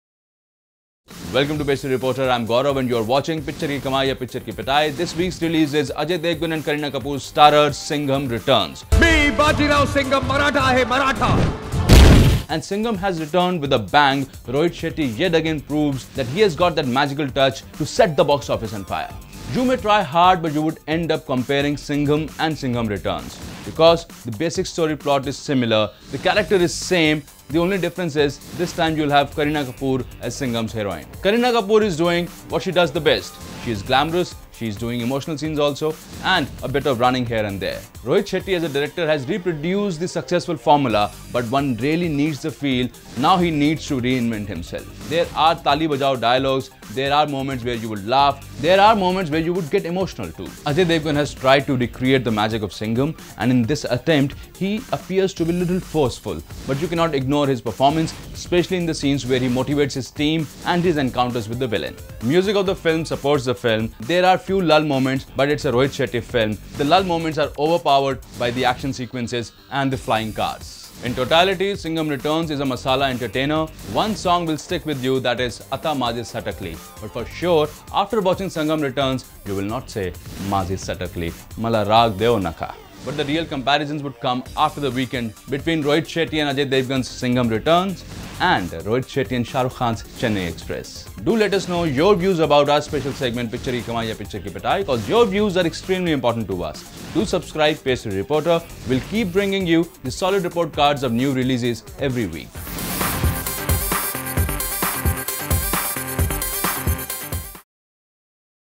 Singham Returns | Full Movie Review | Ajay Devgan, Kareena Kapoor, Amol Gupte
Watch Movie Review  - Singham Returns
Singham Returns is a Bollywood action film directed by Rohit Shetty The sequel to the 2011 film Singham, actor Ajay Devgan reprises his role from the previous film. while Kareena Kapoor Khan plays the female lead.